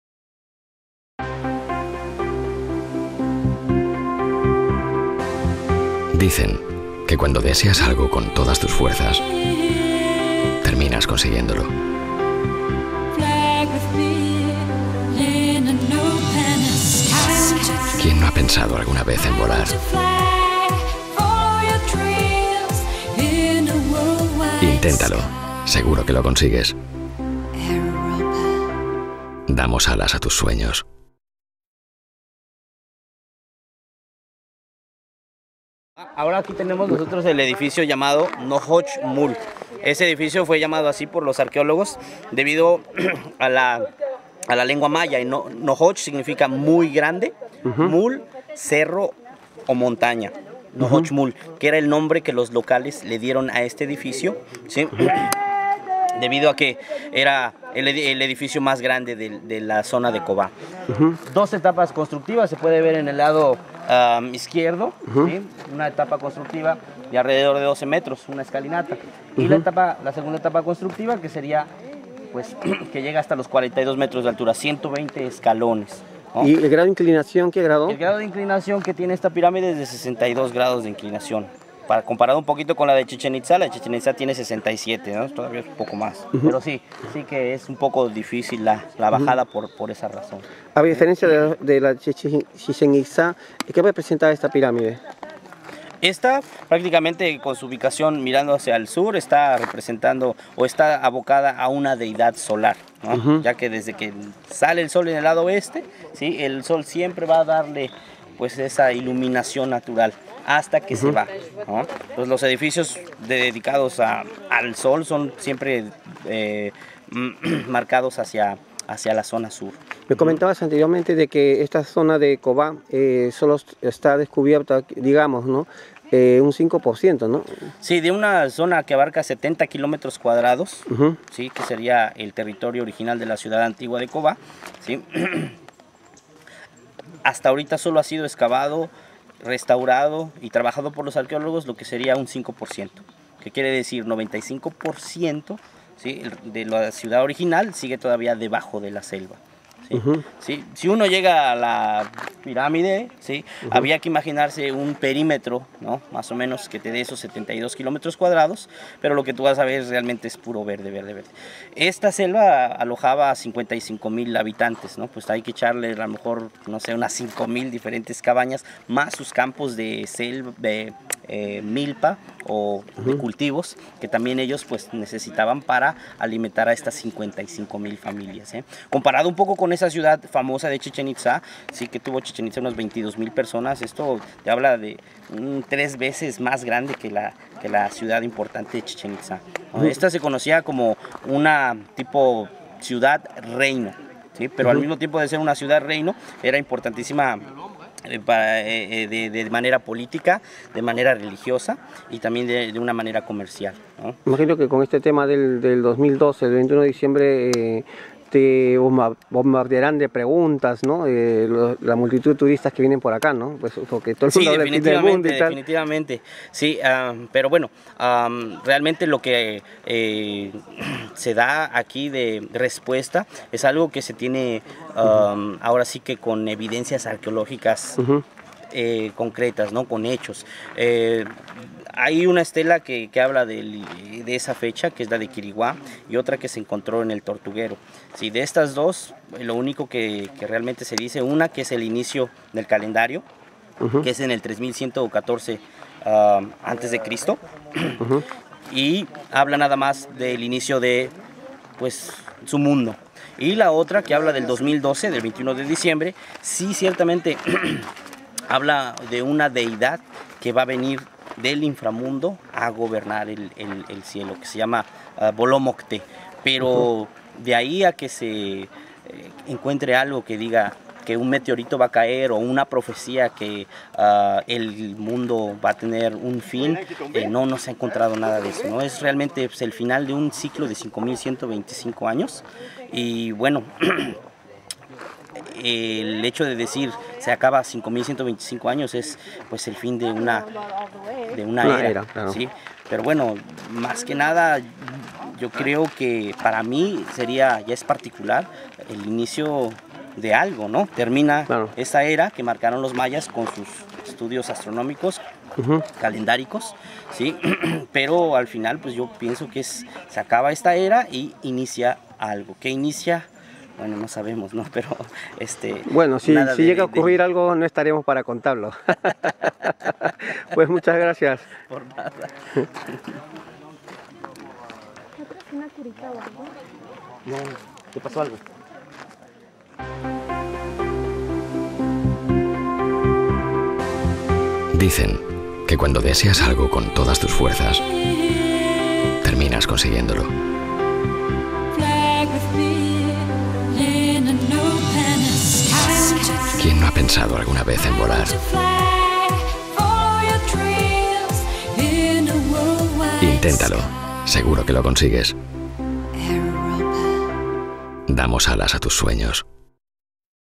COBÁ - PIRÁMIDE DE NOHOCH MUL (Guía maya)
Fampress.com en su periplo por la zona arqueológica de Cobá, entrevista a un guía maya, quien nos detalla las características de la Pirámide maya de Nohoch Mul. Con 42 metros de altura es la más más alta del norte de la península de Yucatán, Desde su cima la vista panorámica de la selva que la bordea es espectacular.